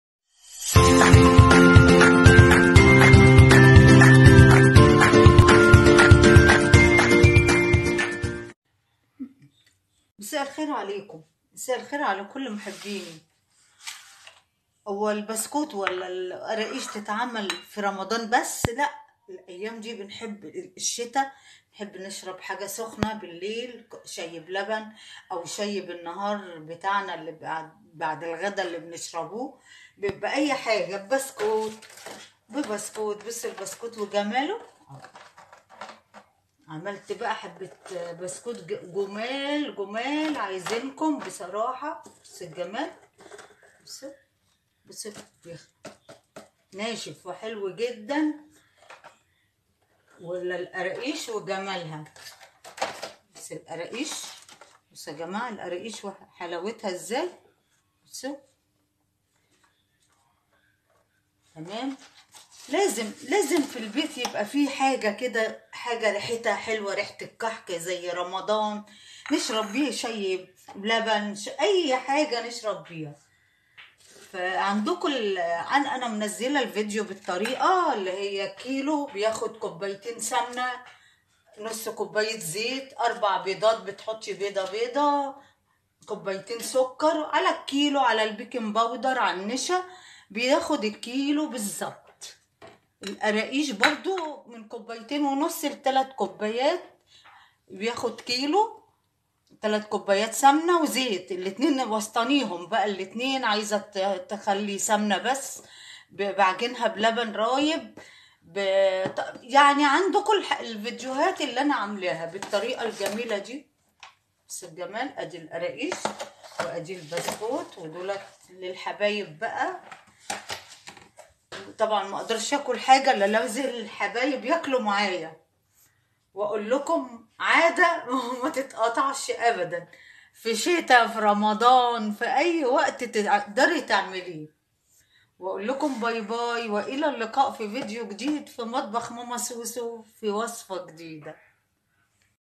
هو مينفعش ‼️البسكويت والقراقيش غير فى رمضان بس تعالوا نعمل فى ايام الشتا احلا كمان ☕☕
البسكويت
كيلو دقيق ملح
٤ بيض فانيليا
٢ كوب سمنه
٢ كوب سكر
ربع كوب زيت
ربع كوب لبن
معلقه كبيره بيكنج بودر.
القراقيش
كيلو دقيق
شمر ويانسون
ملح سمسم
من ٣ كوب سمنه مع زيت
خميره لبن رايب